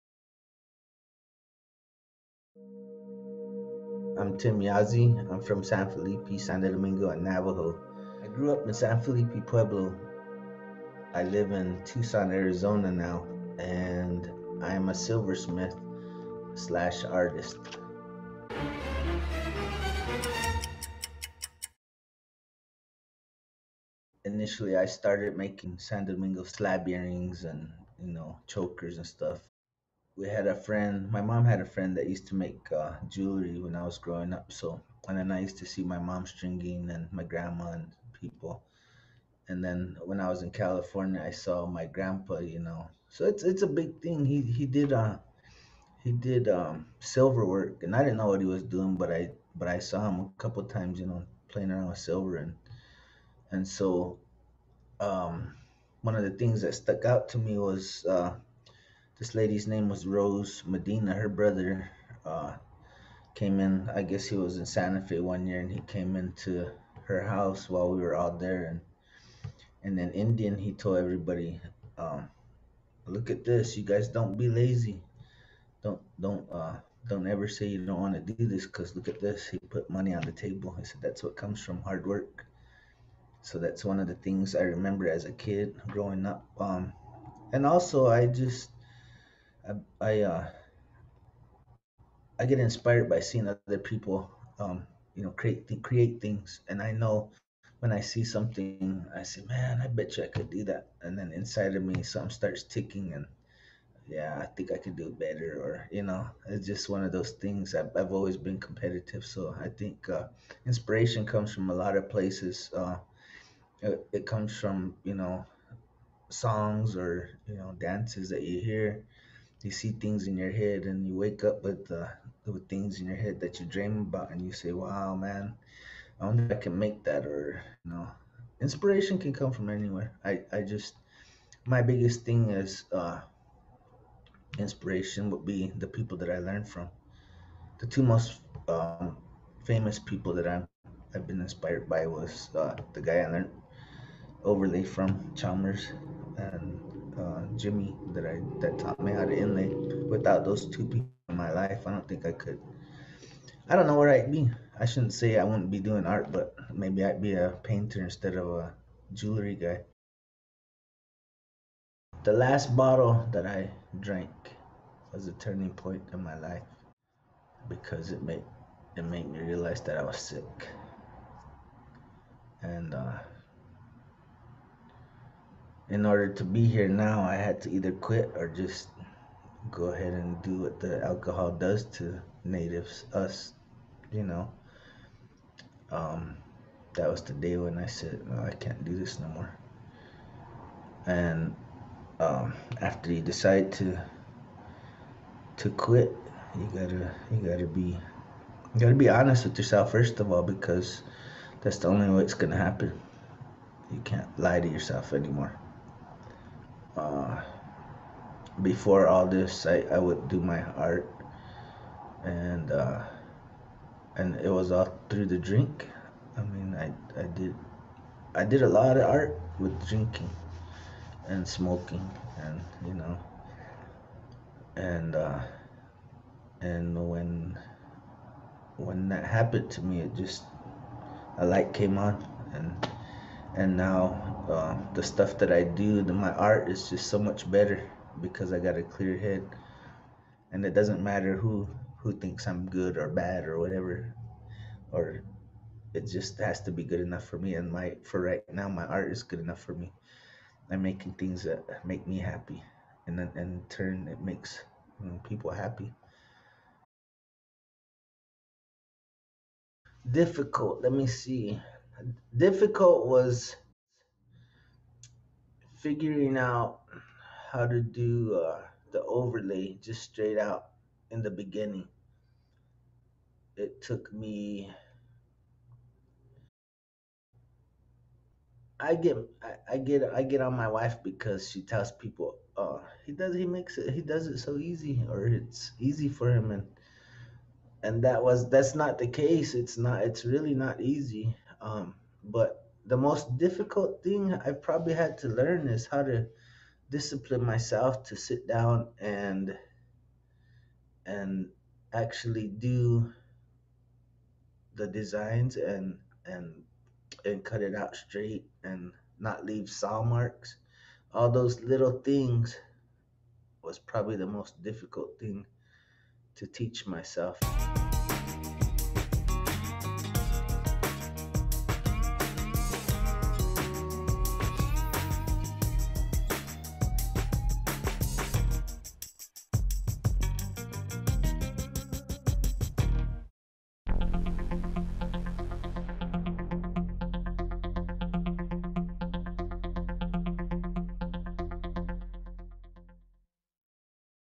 Three Questions With...Tim Yazzie - The Native Silver Son (Pt. 1)
Three Questions With... is a series of interviews with artists who have a working relationship with Wright's Indian Art, located in Albuquerque, NM since 1907.